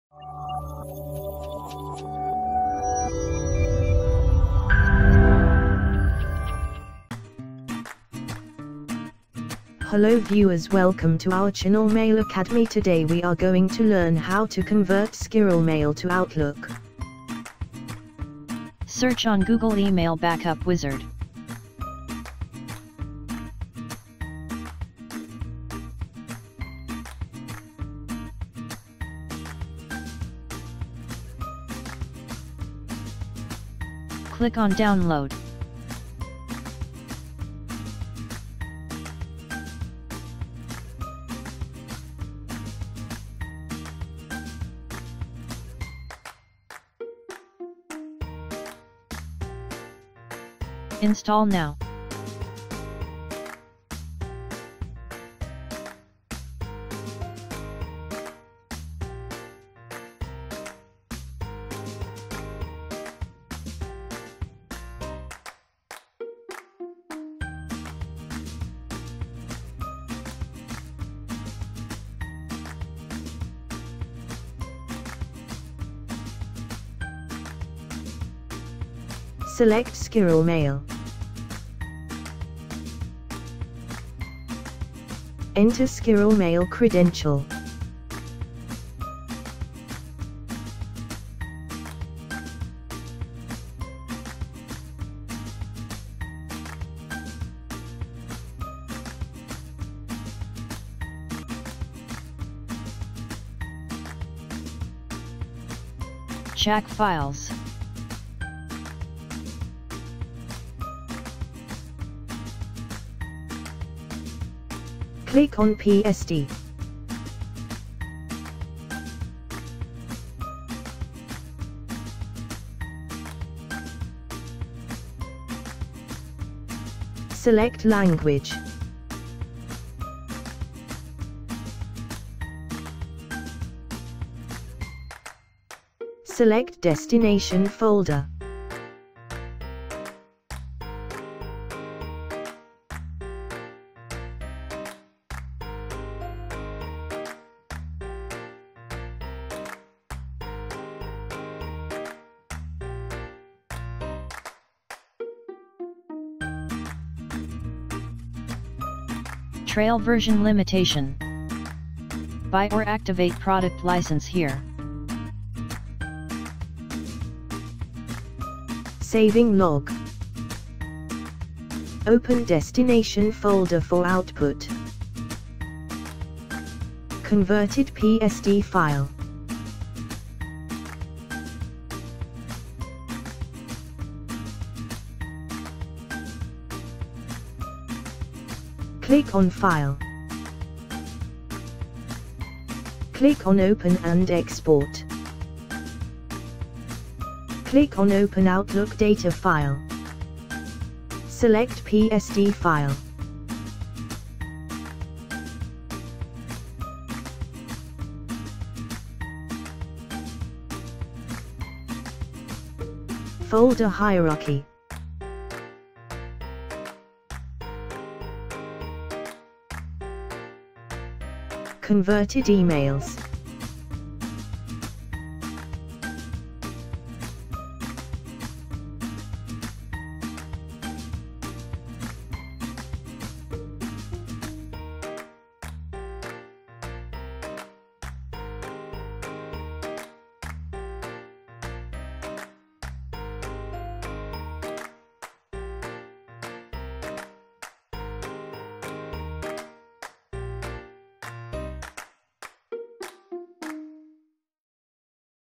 How to Convert SquirrelMail to Outlook 2019, 2016, 2013, 2010 ?
Watch this video tutorial to get detailed information about how to convert SquirrelMail to Outlook ? In this video, Mail Academy team has described the step by step process to export emails from SquirrelMail to Outlook 2007, 2010, 2013, 2016, 2019, etc.

Steps to Export SquirrelMail to Outlook
1. and install it on your Windows Machine.
2. Start the tool and choose SquirrelMail Webmail from email source.
3. Enter SquirrelMail account credentials and press Login button to continue.
4. Check SquirrelMail folders that you want to convert and choose PST (Personal Storage Table) as saving format.
5. Users can change the language of resultant PST file in Dutch, French, German, Spanish, Swedish, etc.
6. Select destination folder and press Backup button to start SquirrelMail to Outlook conversion.
7. After completion of the task, the tool will show a message of completion.
8. Get resultant PST file from destination path.
9. Run MS Outlook 2019, 2016, 2013, 2010 and choose Open Outlook Data File option. Browse PST file from computer and import it to Outlook.

SquirrelMail to Outlook Converter Features
• SquirrelMail to PST Converter asks only SquirrelMail account credentials for completing the task.
• SquirrelMail to Outlook Converter creates Unicode PST file which is compatible with all latest Outlook editions.
• This application preserves all email items, components, attachments, signature, subject, to, cc, bcc, etc. during export SquirrelMail to Outlook.
• The tool supports all latest as well as previous Windows OS editions successfully such as Win 10, 8.1, 8, 7, Vista, XP, Win Server 2019, 2016, 2012, 2008, 2003, etc.